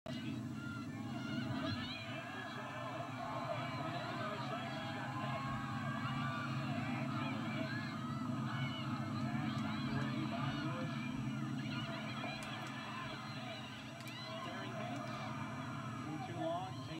Women's DI Soccer: ND vs. Santa Clara, 2006
Amanda Cinalli leads counter attack with second effort